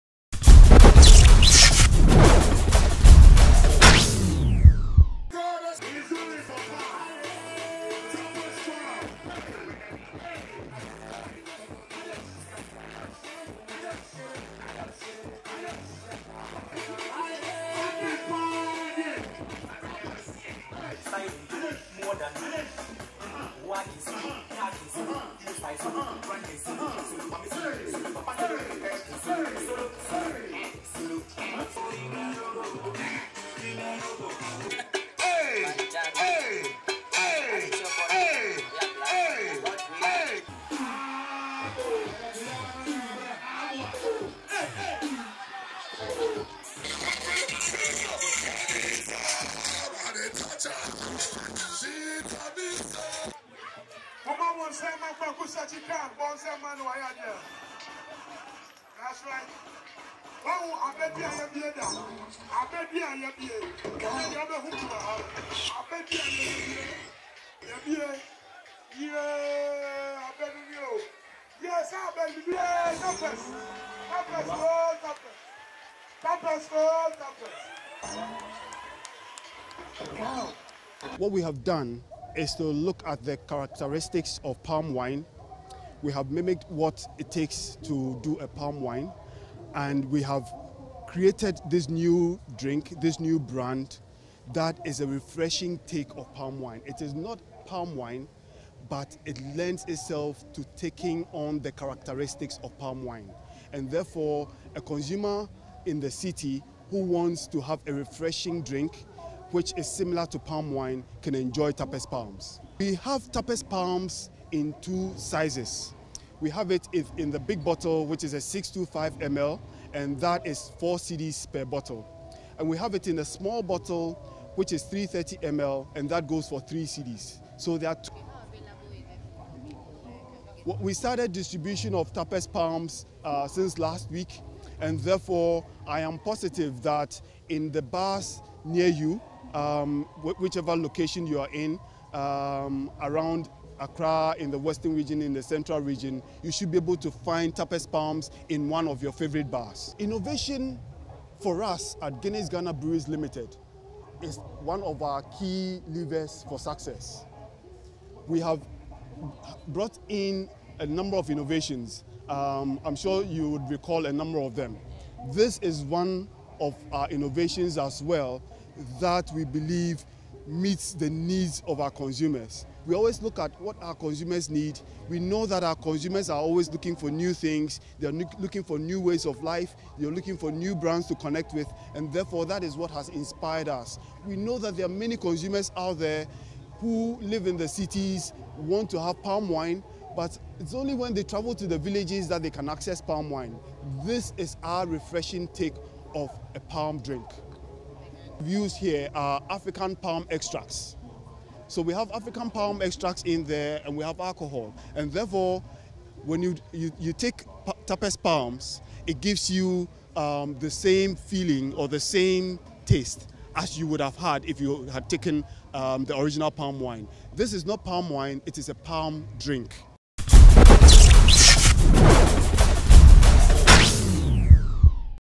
Guinness Ghana Breweries Limited Launch Tappers Palms | Pulse Events
Guinness Ghana Breweries Limited on Sunday, November 27, 2016 launched TAPPERS PALMS, a crisp, refreshing, sparkling drink at the Labadi Beach in Accra.